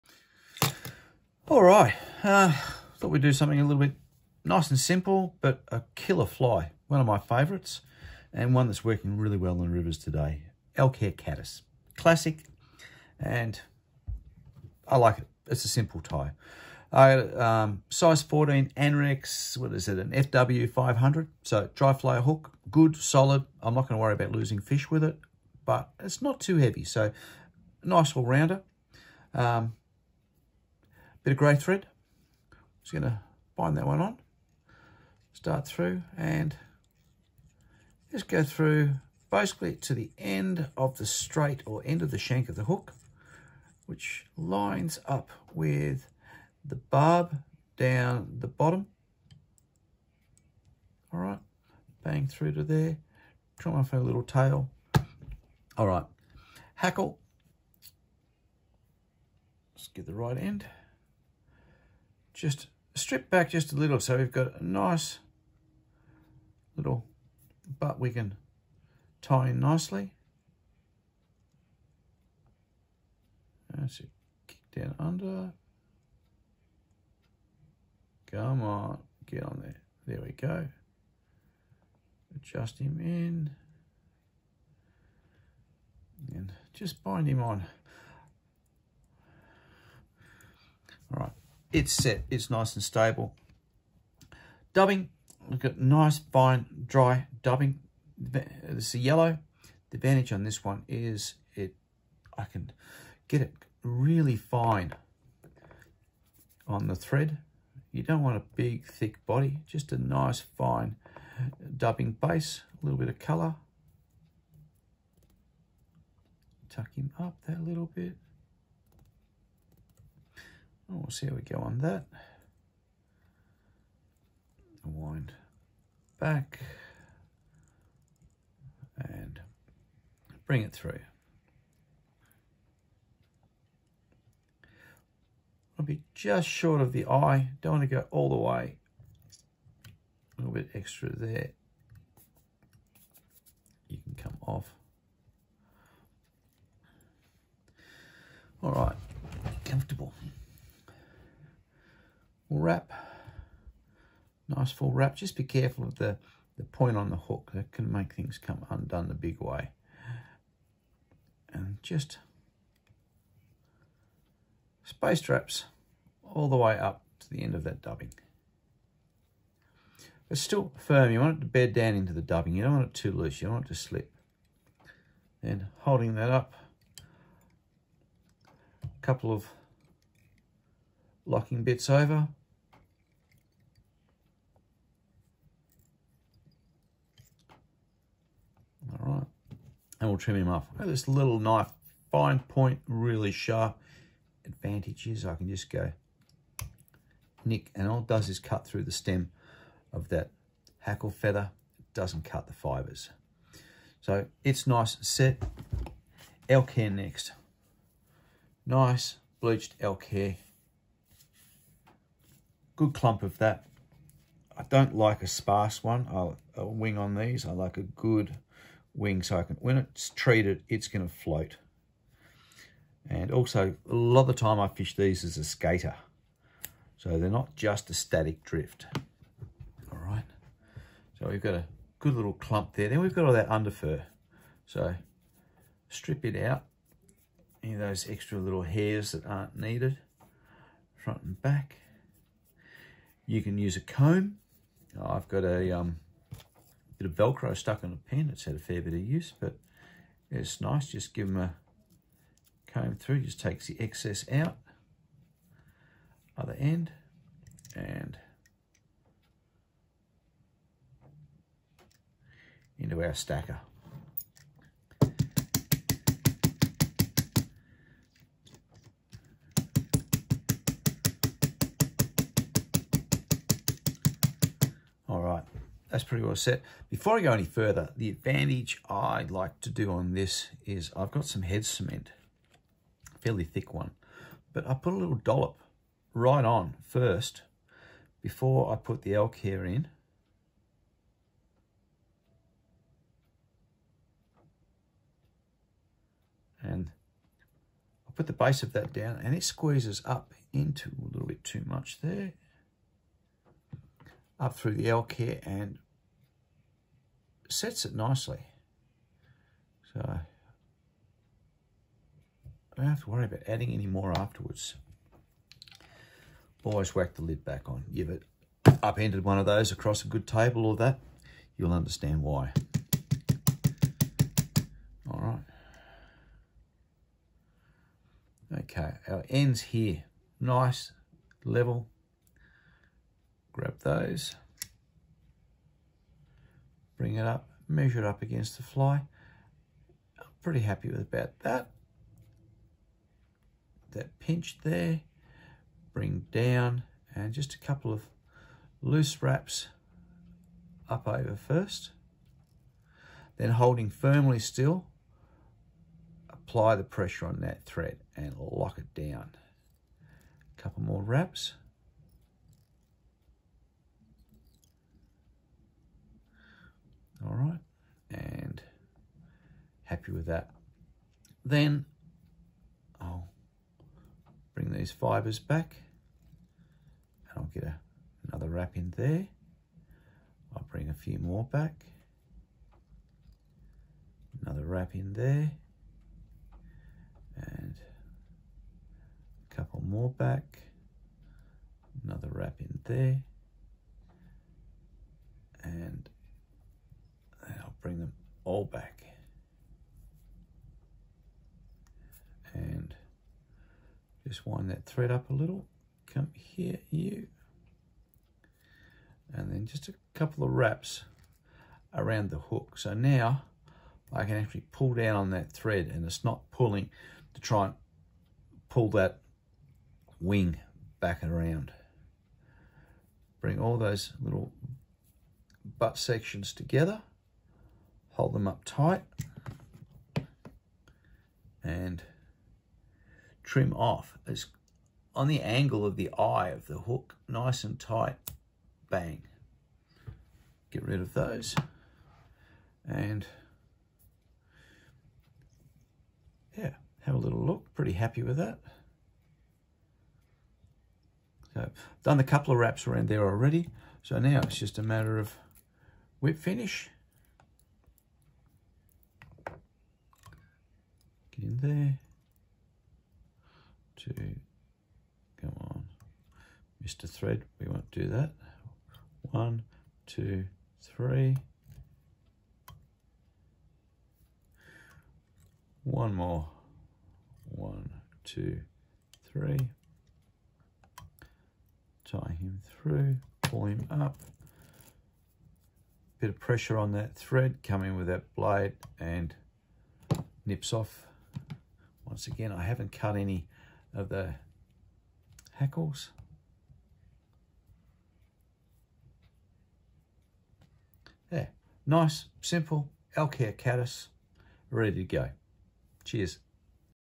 Tying the Elk Hair Caddis Fly
Scottie’s easy method of tying the classic Elk Hair Caddis